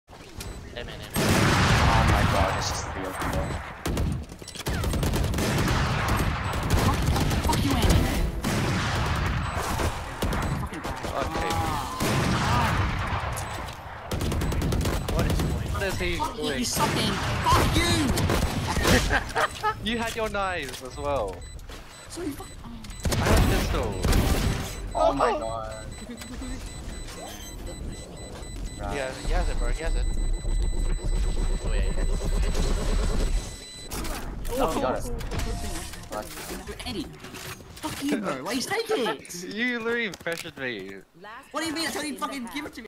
ur team when u trying to clutch up......
team stole each others gun then they started fighting each other, the person that stole the gun whiffed so hard and died at the end